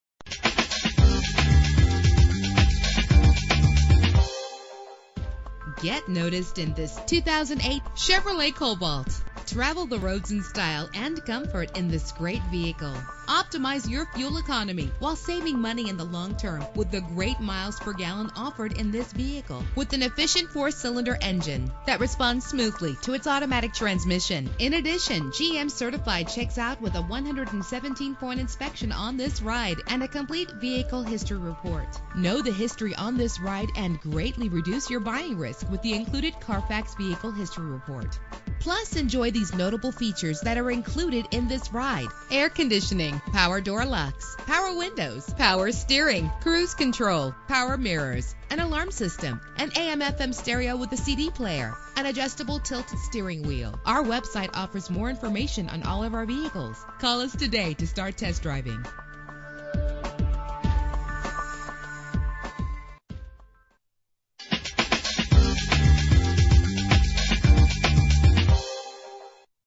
2008 Chevrolet Cobalt - PEARLAND TX
Year: 2008
Make: Chevrolet
Model: Cobalt
Trim: LT
Engine: 2.2 liter inline 4 cylinder 16 valve
Transmission: Automatic with Overdrive
Color: Red
Mileage: 40638
Address:
5719 BROADWAY
PEA